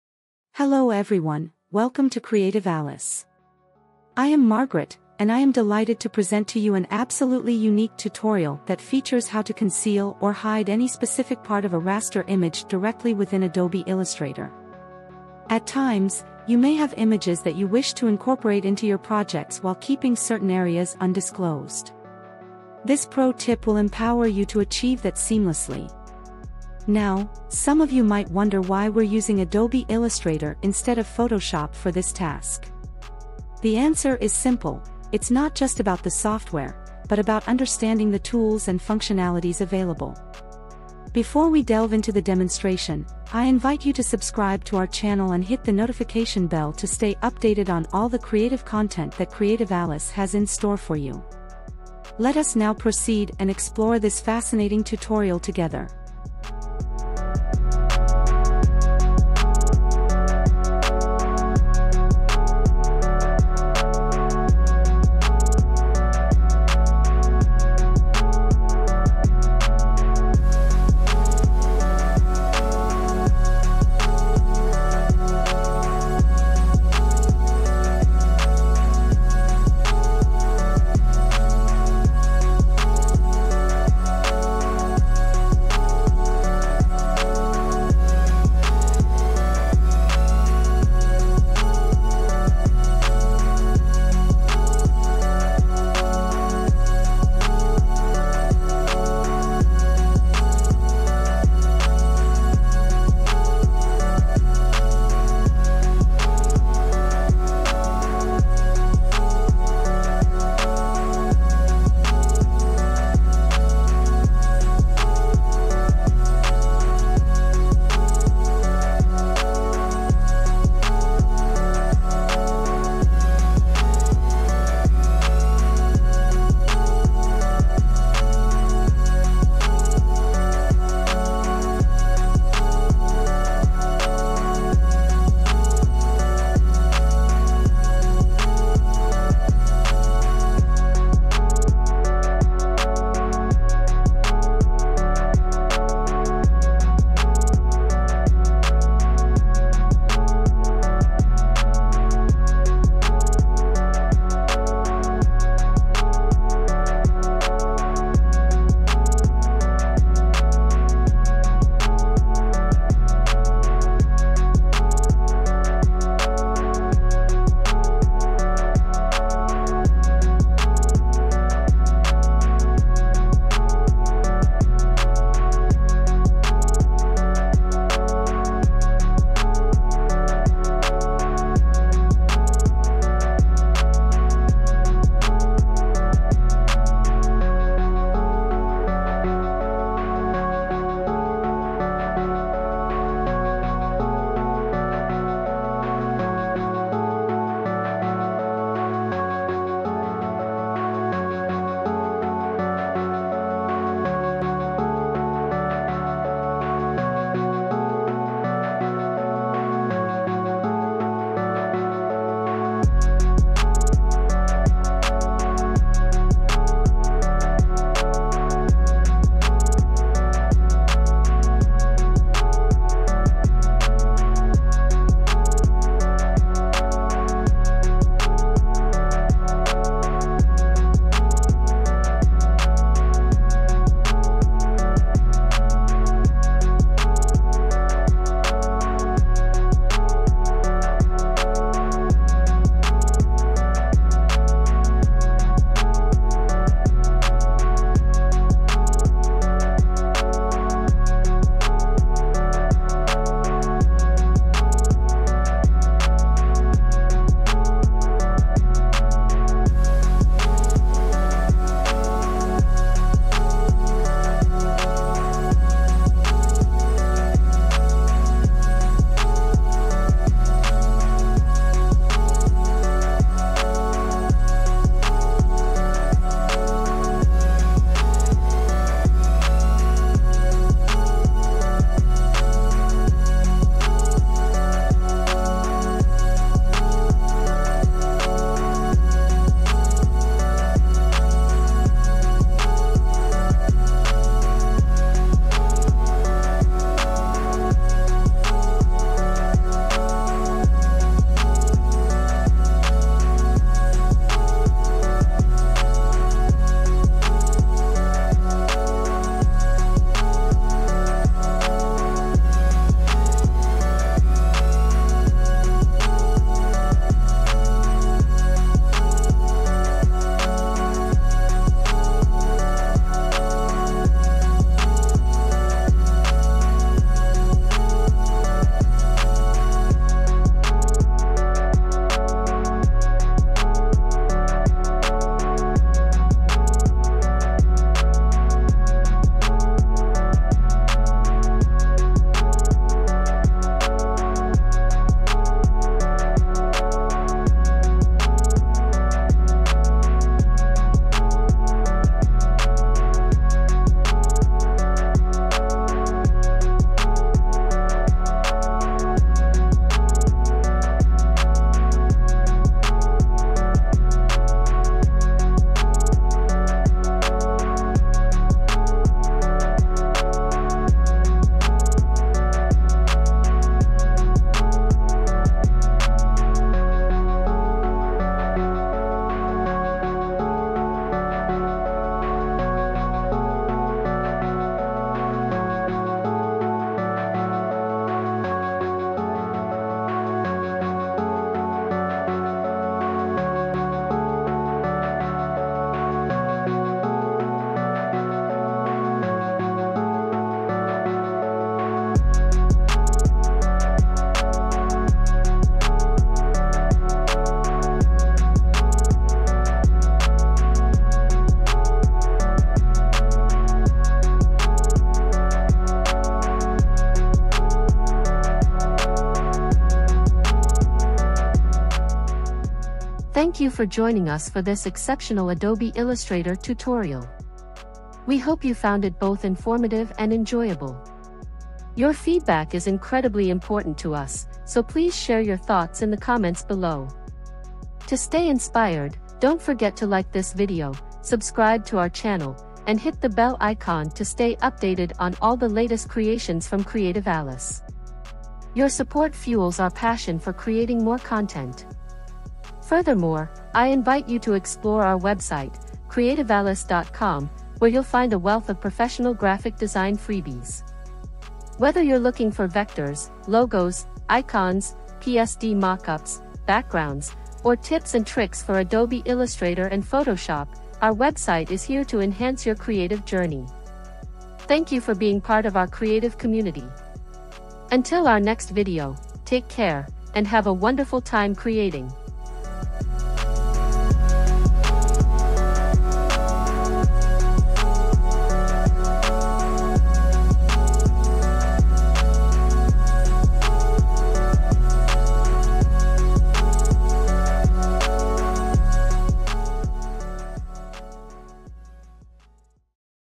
How to Blur or Mosaic Any Part of Images | Illustrator Tutorial
Learn how to conceal or hide any part of a raster image in Adobe Illustrator using blur or mosaic effects in this step-by-step tutorial. Perfect for designers looking to add privacy or creative effects to their images. Follow along with @Creativealys and enhance your Illustrator skills today!

📌 In this video, you'll learn:

➜ How to apply blur effects to specific parts of an image
➜ Creating mosaic patterns to hide details
➜ Tips for working with raster images in Illustrator

00:00 Intro
01:00 Method one to conceal part of an image
01:22 Declare specific area of an image to conceal
03:04 Method two to conceal part of an image
05:19 Method three to conceal part of an image
07:02 Outro